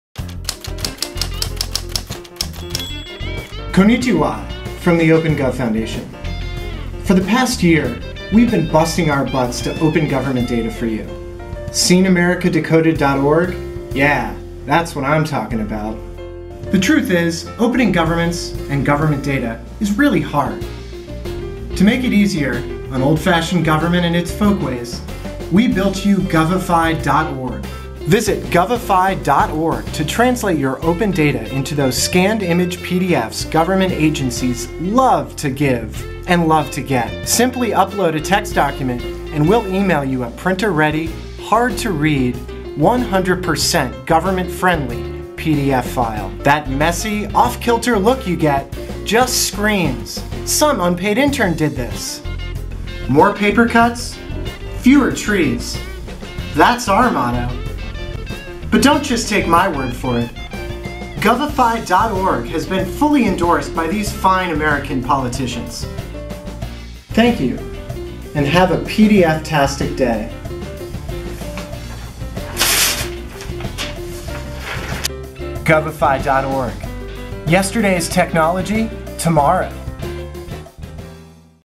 Govify.org: Yesterday's Document Technology, Today!
For the past year, we at The OpenGov Foundation have been busting our butts to open government data for you.  Seen  Yeah, that's what I'm talkin' about.

Opening data and governments is hard, thanks to decades of boondoggles, bad technology and bureaucratic stick-in-the-muds.  To make it easier on old-fashioned governments, we built Govify.org.

Visit Govify.org to translate your open data into those scanned-image PDFs government agencies love and understand.  Simply upload a text document and we'll email you a printer-ready, hard-to-read, government-friendly PDF.

That messy, off-kilter look just screams, "Some unpaid Intern did this!"

More paper-cuts, fewer trees.  That's our motto.

And don't just take my word it: Govify.org is proudly endorsed by these great Americans.

Thank you and have a PDF-tastic April Fool's Day!